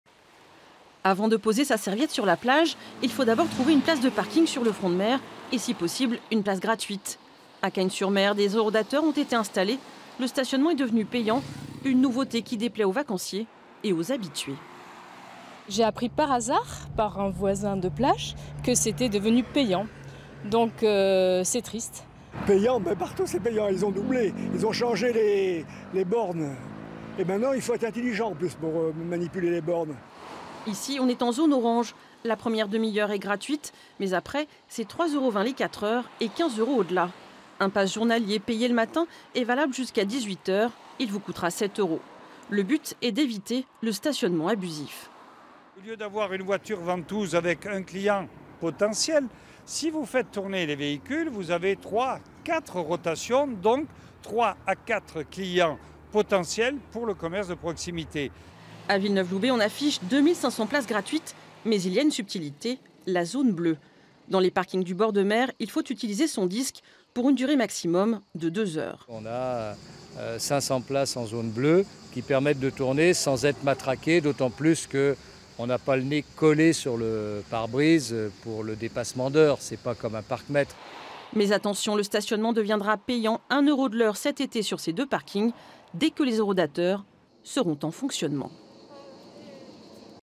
Cagnes-sur-Mer : le stationnement devient payant sur le front de mer
Mauvaise nouvelle pour les vacanciers : sur le front de mer de Cagnes-sur-Mer, le stationnement devient payant. Mais ce n'est pas le cas de toutes les communes.

Intervenants : Louis Nègre (Maire de Cagnes-sur-Mer), Lionnel Luca (Maire de Villeneuve-Loubet)

Equipe : Aline Métais, Denis Pardanaud, Johann Gross